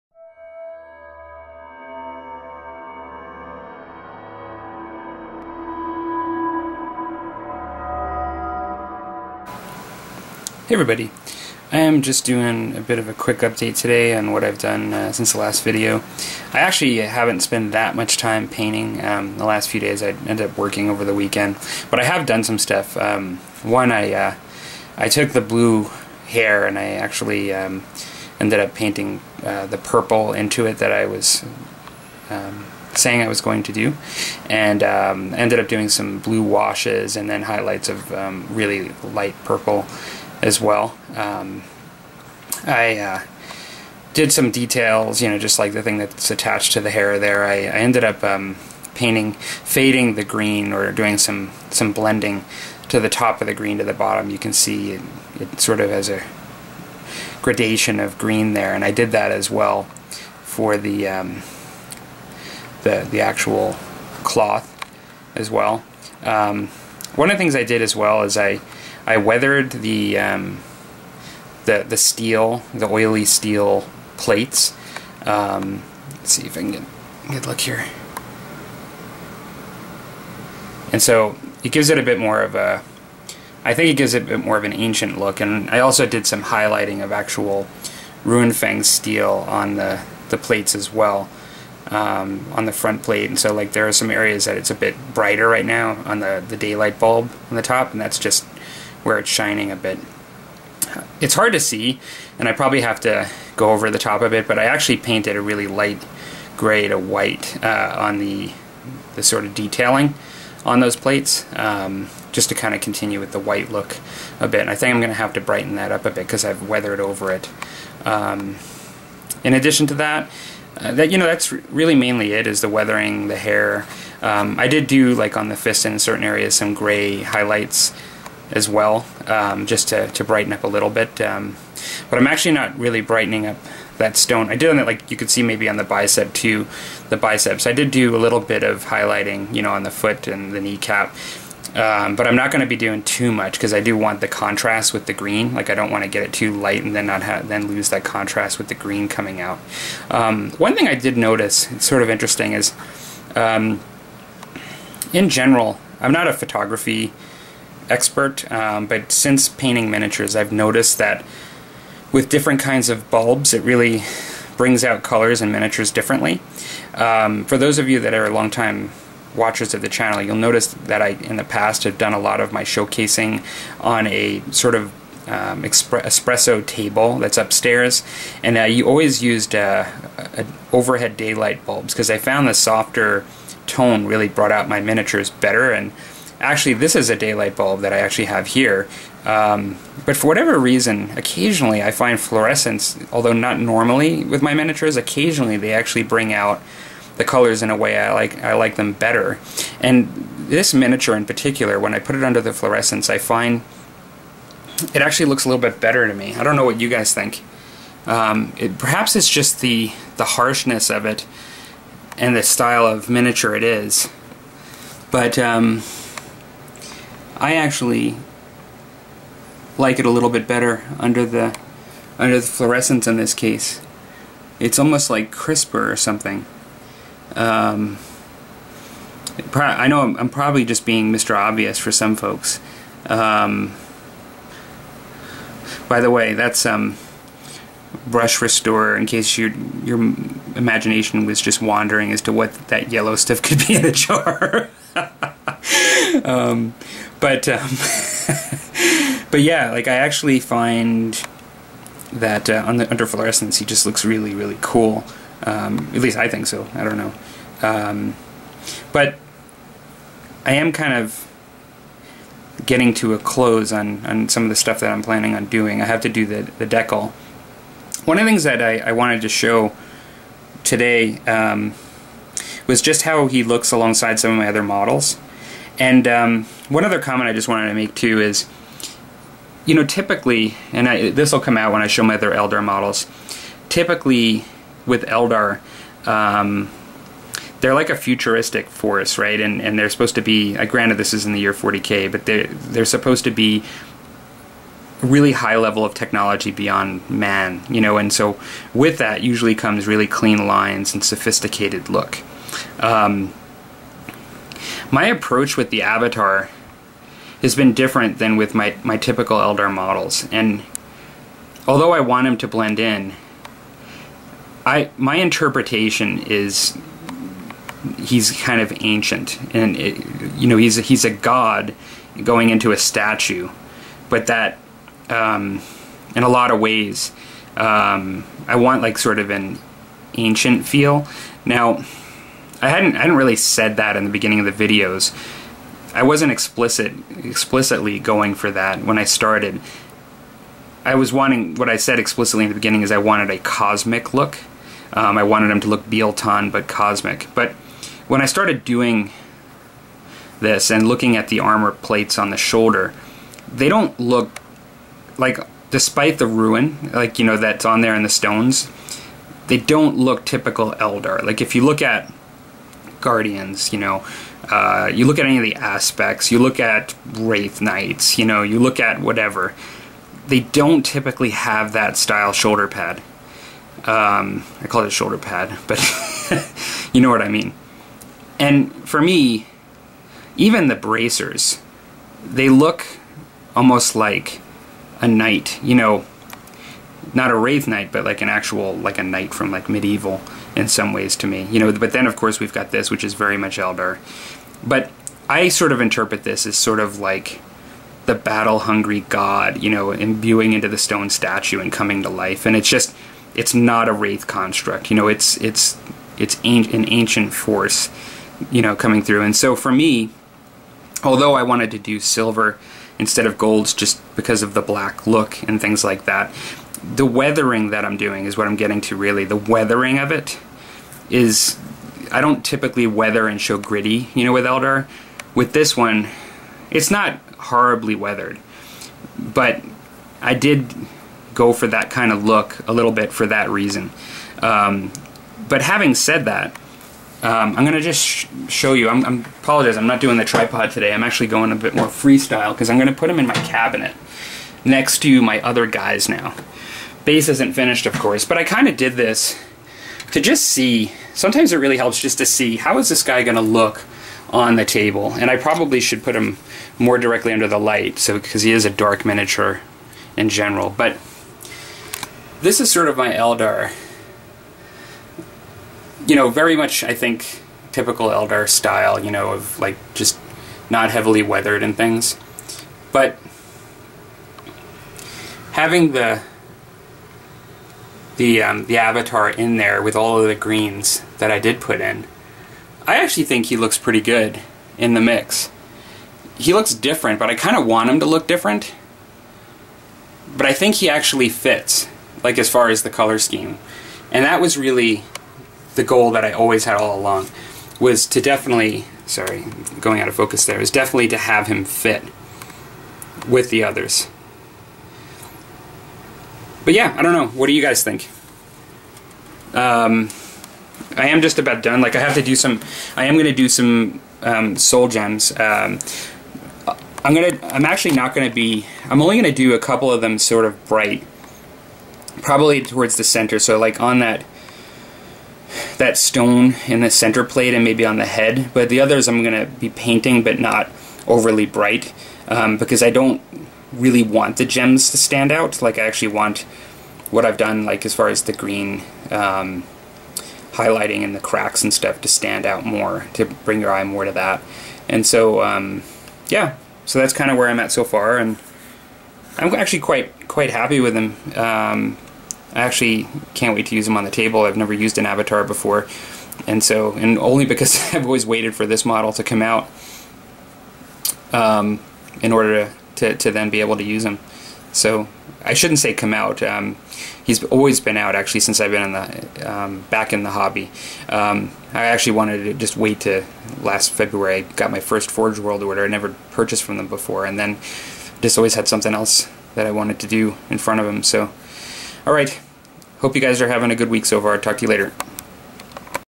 NNYPC: Eldar Avatar WIP- 4
An update on the NNYPC Eldar avatar I am painting